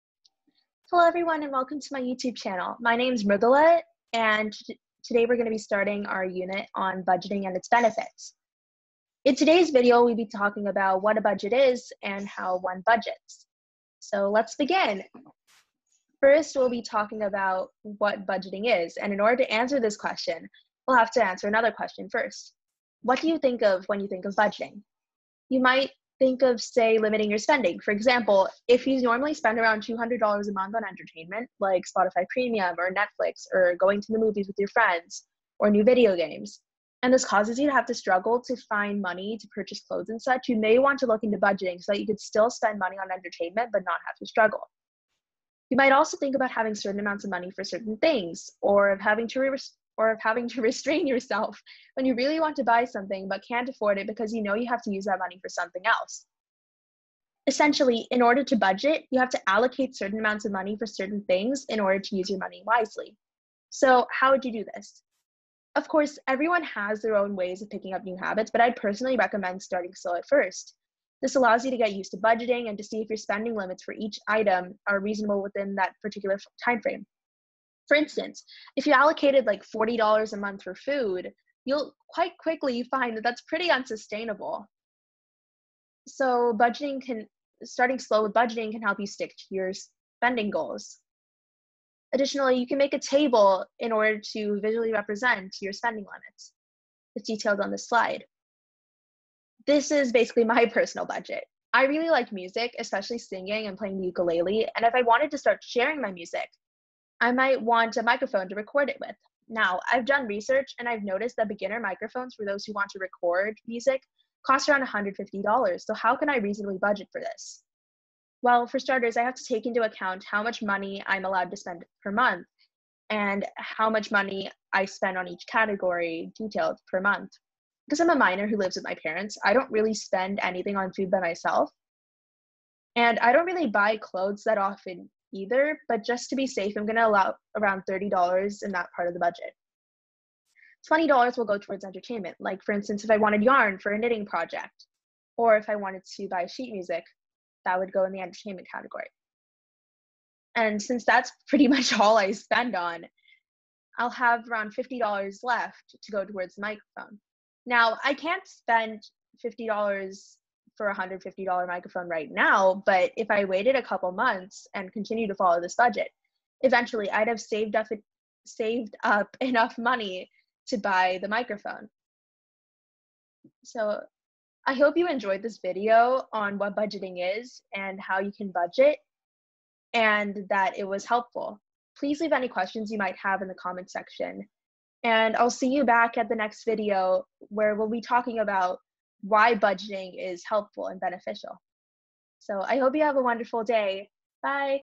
What is budgeting and how do I budget?
Hi guys! My name is Mrudula and I'm a high schooler in the Bay Area who's really interested in finance. :) I hope this video is helpful! Please make sure to leave any questions you might have in the comment section! :)